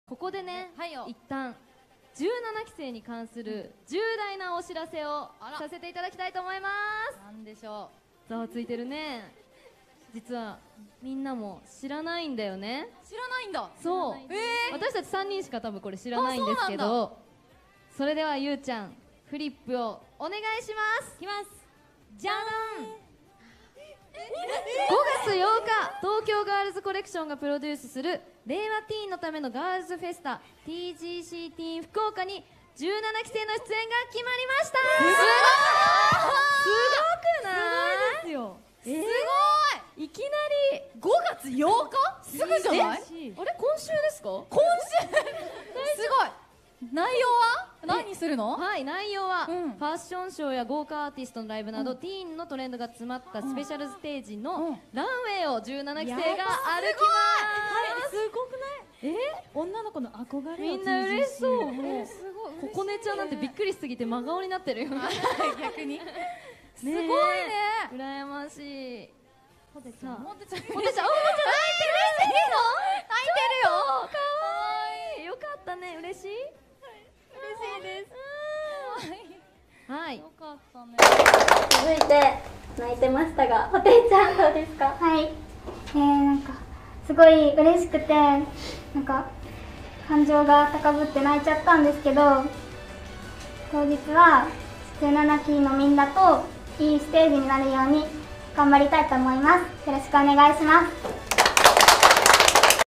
[ENGSUB] 17th gen's surprise appearance on "TGC teen 2022 Fukuoka" made Hotei Moka burst out crying
Hotechan is so precious 🥺